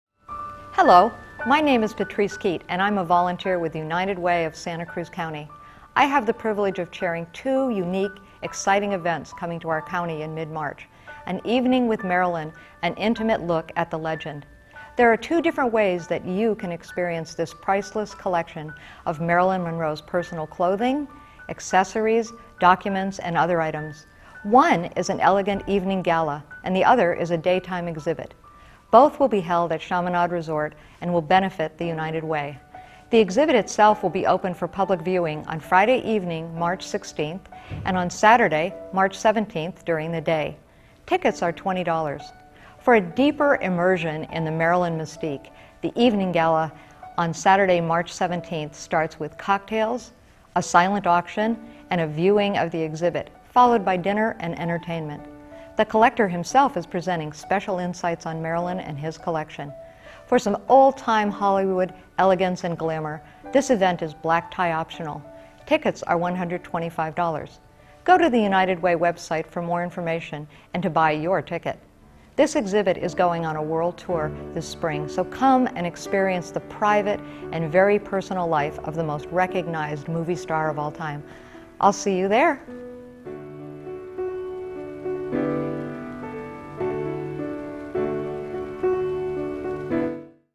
United Way of Santa Cruz County "An Evening with Marilyn: An Intimate Look at the Legend"
There are two different ways that you can experience this priceless collection of Marilyn Monroe's personal clothing, accessories, documents and other items. One is an elegant evening gala and the other is a daytime exhibit. Both will be held at Chaminade resort and will benefit the United Way.
 The exhibit itself will be open for public viewing on Friday evening  March 16th and on Saturday March 17th during the day. Tickets are $20.
For a deeper immersion in the Marilyn mystique, the Evening Gala on Saturday March 17th starts with  cocktails, a Silent Auction and a viewing of the exhibit followed by dinner and entertainment. The collector himself is presenting special insights on Marilyn and his collection.  For some old time Hollywood elegance and glamour, this is event is black tie optional. Tickets are $125.